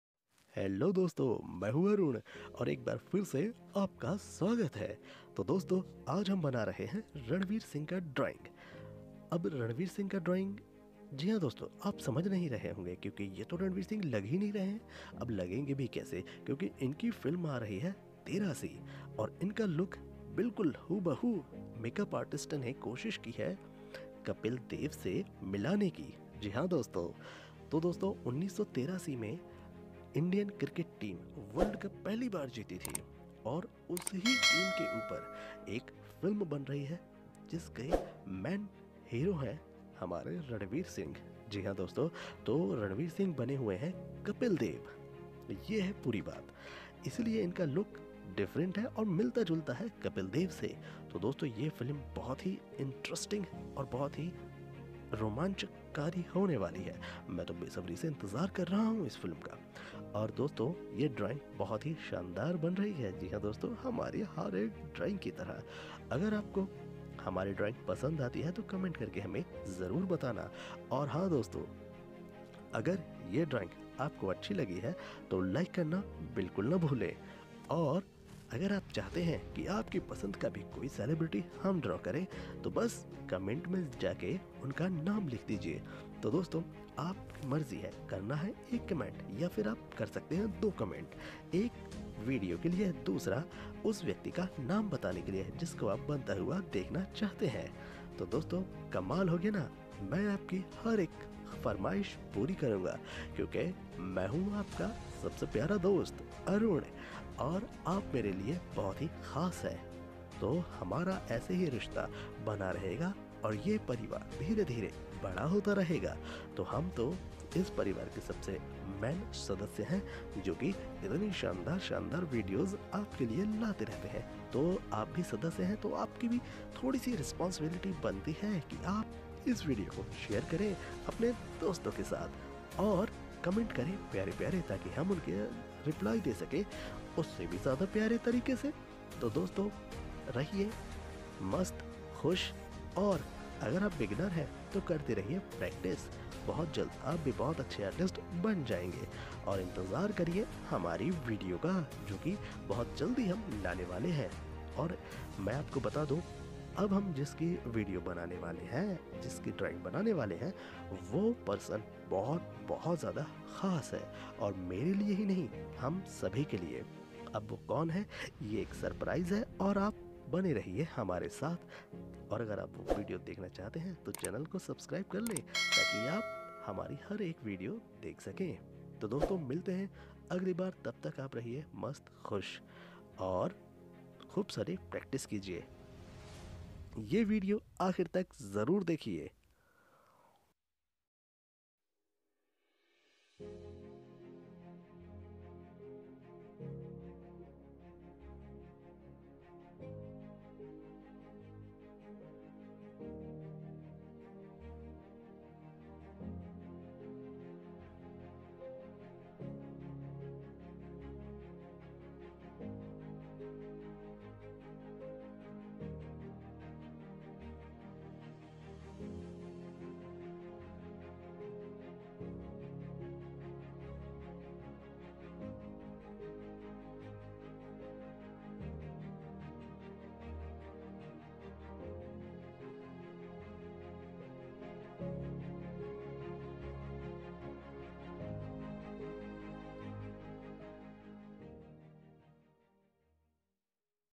83 movie hero drawing / 83 movie hero ranveer singh drawing / ranveer singh sketch
hello, friends welcom to arun art 7
friends i draw today ranveer singh new look movie of 83, this channel is dedicated by  my beginner artist friend's,
 my  all drawing videos is so usefull and learn easy drawing & sketch ranveer singh 83 movie look,  i use pancil is hb,  4b, 8b, 10b, and...